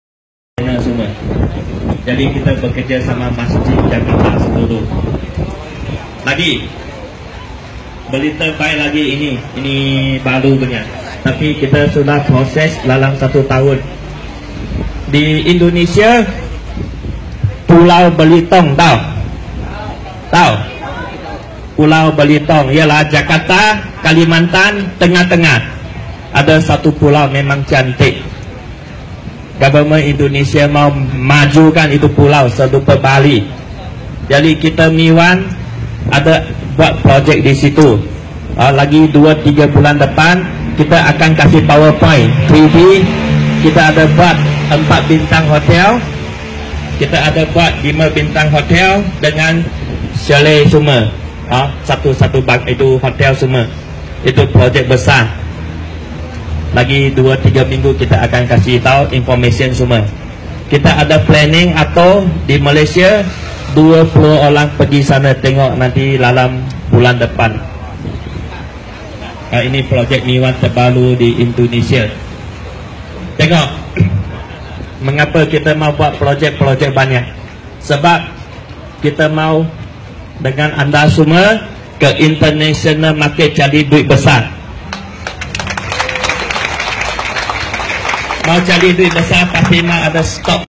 MI1 GLOBAL akan melancarkan HOTEL BERTARAF 4 & 5 BINTANG di Indonesia
❗❗❗BERITA GEMPAR ❗❗❗

Dato Dr. KC LIM menyampaikan berita gembira untuk semua para MYMI1 members di Mi1 Station semasa majlis open house hari raya pada 23/7/2016.

Mi1 Hotel bertaraf 4 & 5 Bintang akan dilancarkan di Pulau Belitung Indonesia tidak lama lagi kerana syarikat kita mahu pergi ke peringkat Global .

Satu berita yang sangat mengembirakan . Semakin hari kita semakin jelas flow company makin hari semakin berkembang .

Fight Never Surrender